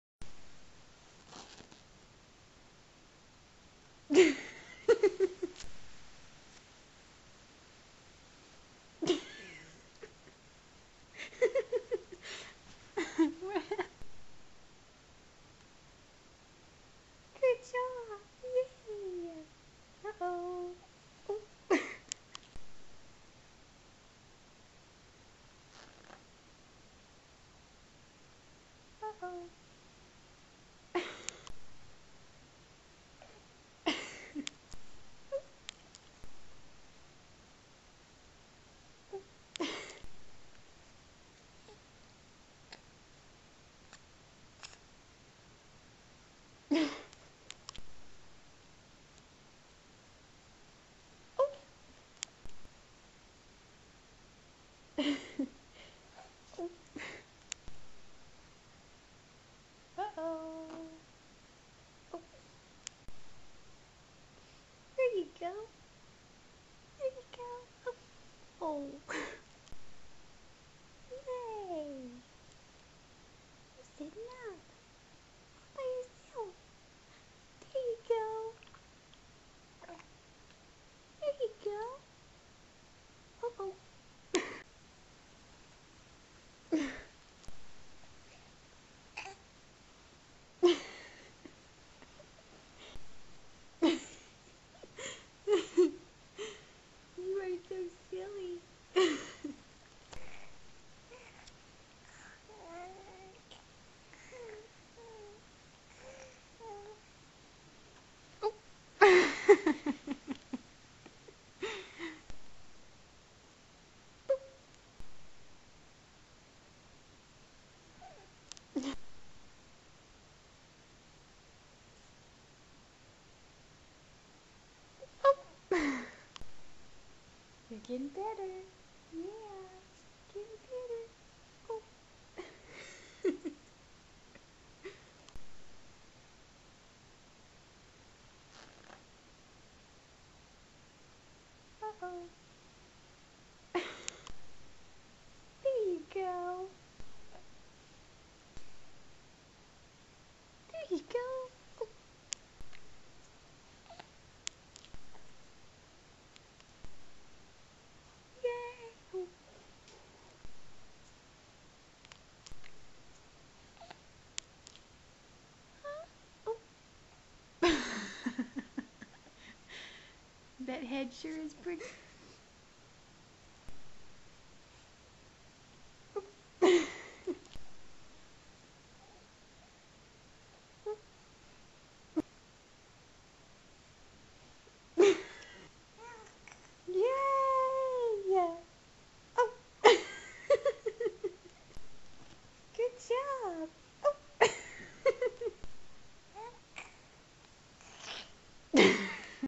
Is it Possible?
This Baby is really trying hard to get through this phase. Its really very cute video featuring a baby attempting to make his first.